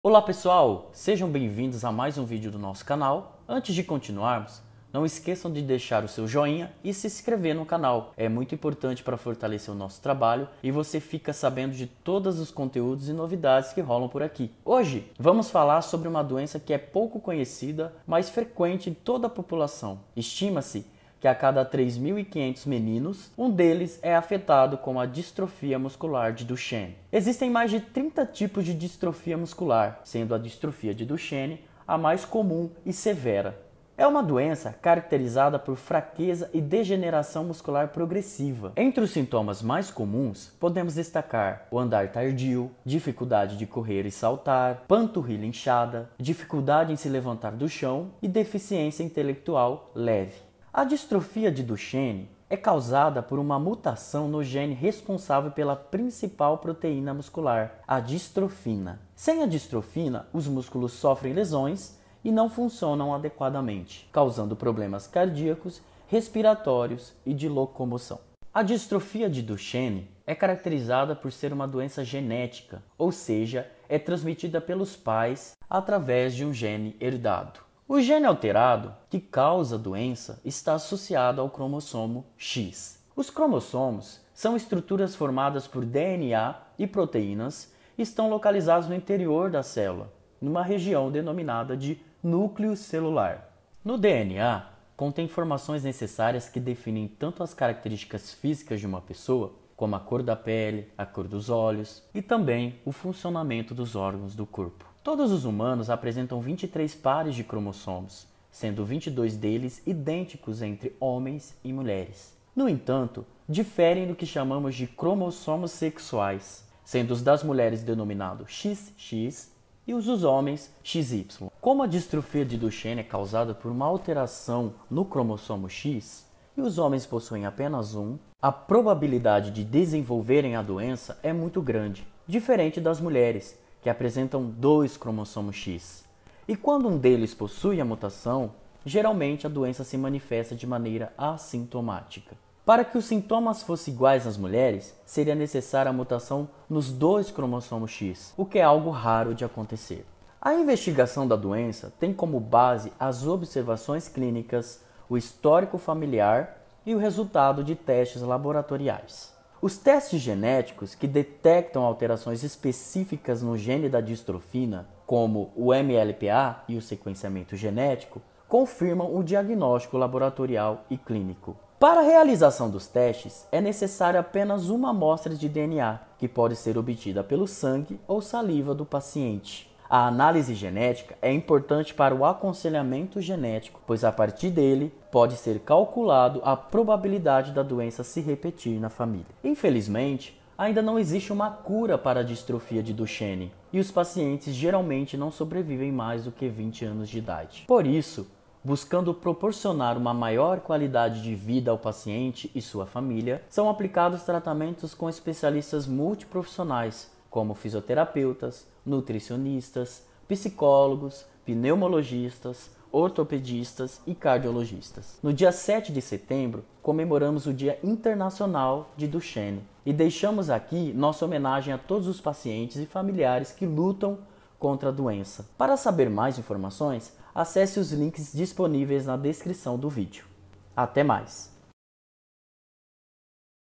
7 de Setembro - Dia Internacional de Conscientização sobre Distrofia Muscular de Duchenne
Nesse vídeo falamos um pouquinho sobre a Distrofia Muscular de Duchenne (DMD), uma doença que causa fraqueza progressiva dos músculos, afetando 1 em cada 3500 meninos.

Vídeo foi adaptado de post do Instagram @ppgbef.unifesp